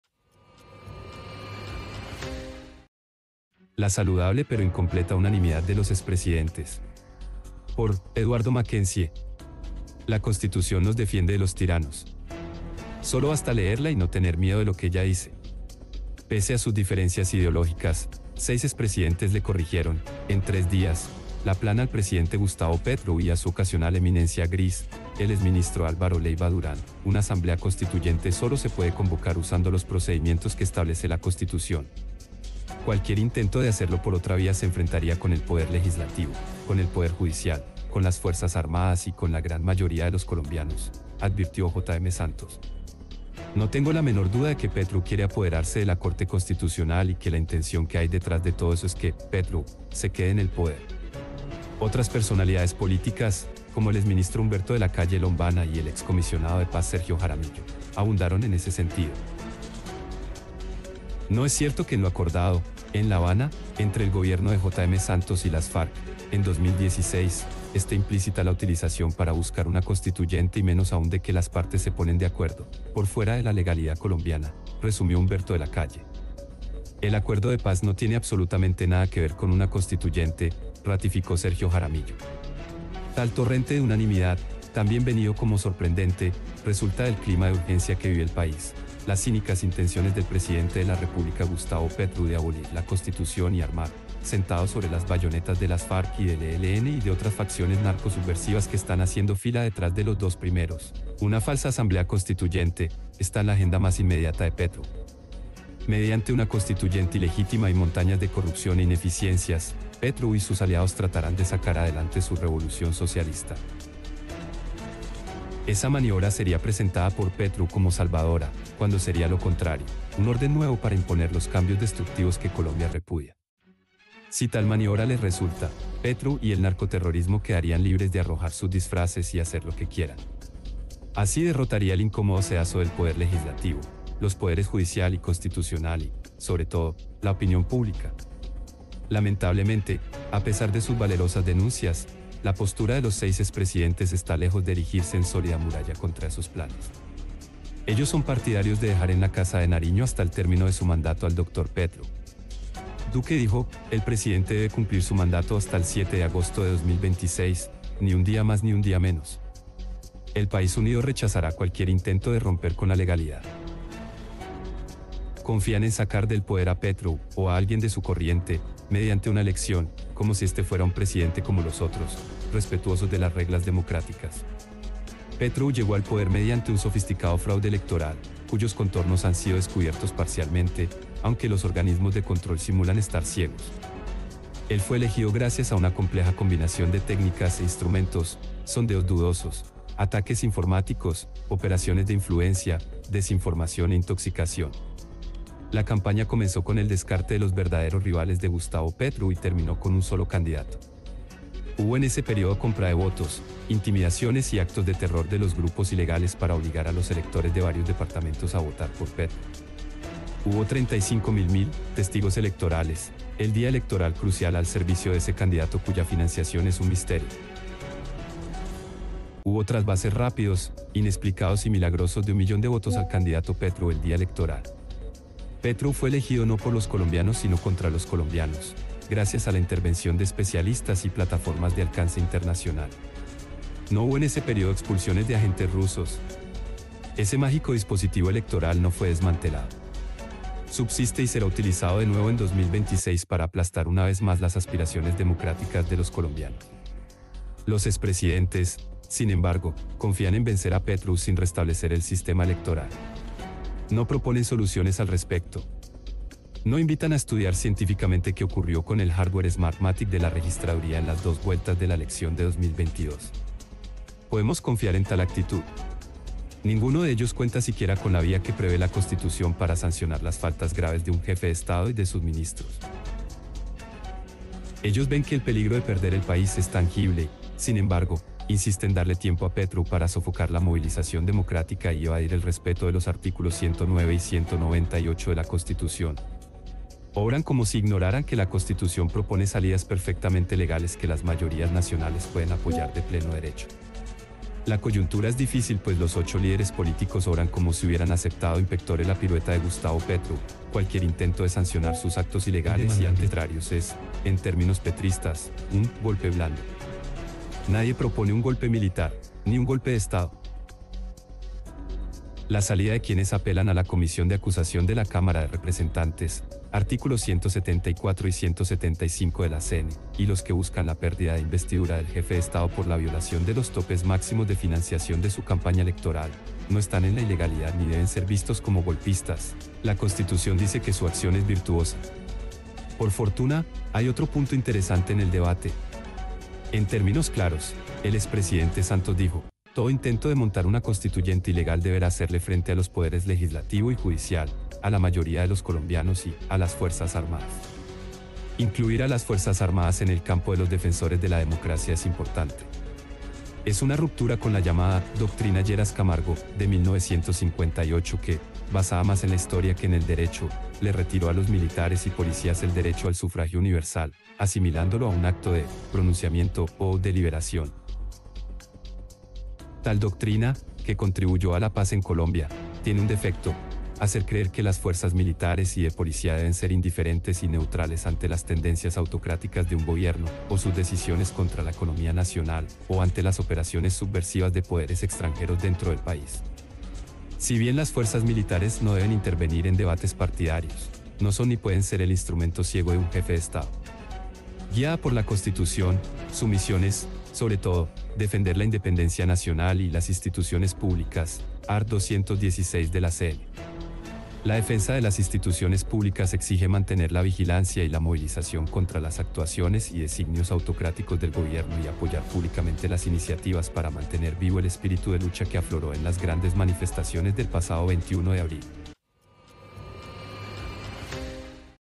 🔴 LA SALUDABLE PERO INCOMPLETA UNANIMIDAD DE LOS EXPRESIDENTES
Pese a sus diferencias ideológicas, seis expresidentes le corrigieron, en tres días, la plana al presidente Gustavo Petro y a su ocasional eminencia gris, el exministro Álvaro Leyva Durán: “Una Asamblea Constituyente sólo se puede convocar usando los procedimientos que establece la Constitución. Cualquier intento de hacerlo por otra vía se enfrentaría con el poder legislativo, con el poder judicial, con las fuerzas armadas y con la gran mayoría de los colombianos”, advirtió JM Santos. “No tengo la menor duda de que Petro quiere apoderarse de la Corte Constitucional y que la intención que hay detrás de todo eso es que [Petro] se quede en el poder”.